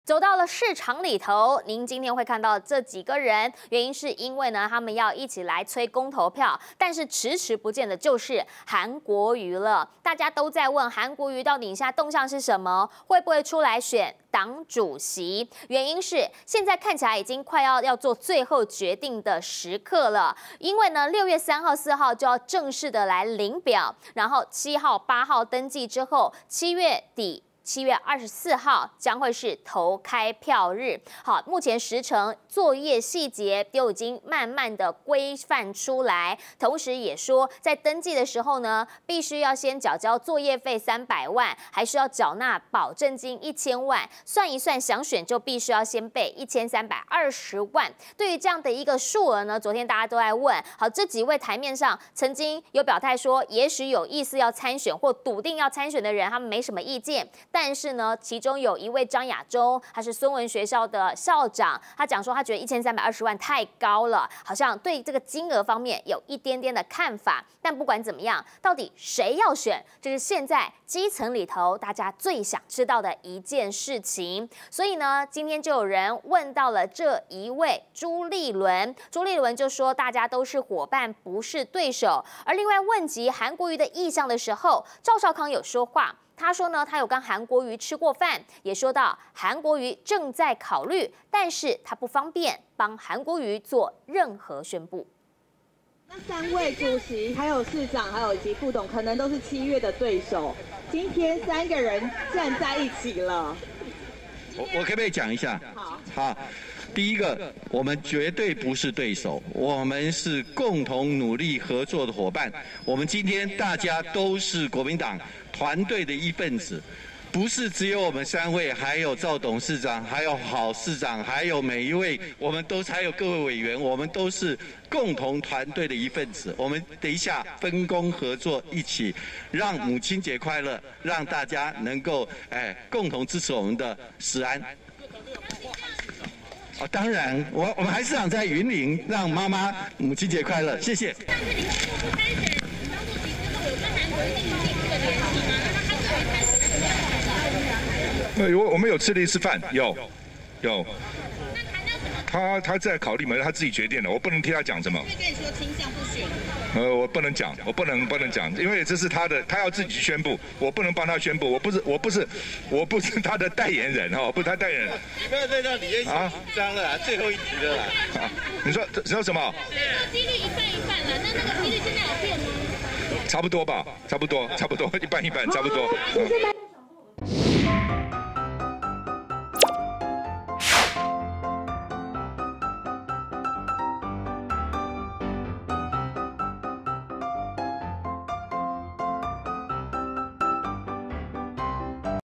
【陳諺瑩報新聞】韓國瑜將參選黨主席？趙少康:韓在考慮中@中天新聞CtiNews 20210508
★支持💪監督的力量在中天！